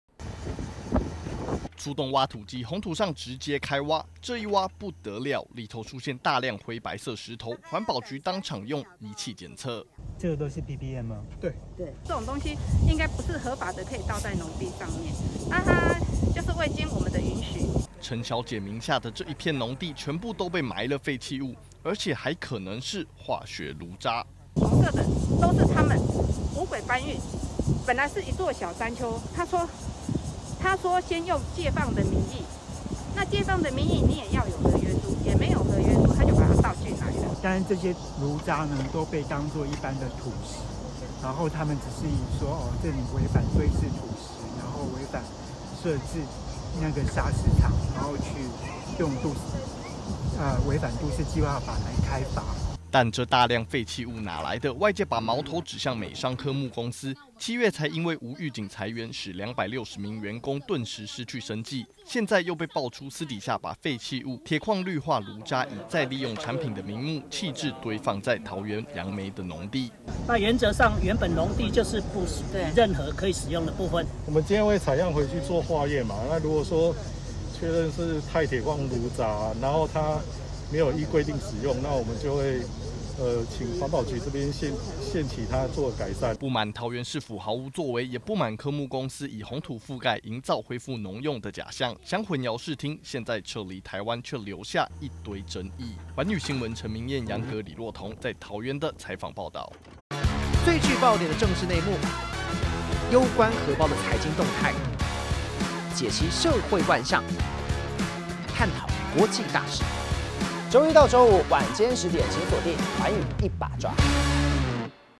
農地非法掩埋廢棄物 美商科慕再爆爭議｜寰宇新聞
美商科慕公司7月才爆出 無預警裁員的勞資風波，8月又被爆出，私下將廢棄物棄置，堆放在楊梅農地，地主相當不滿，甚至懷疑被傾倒的，是化學爐渣，"鐵礦氯化爐渣"。22日桃園環保局出動挖土機，直接派人開挖採樣做檢測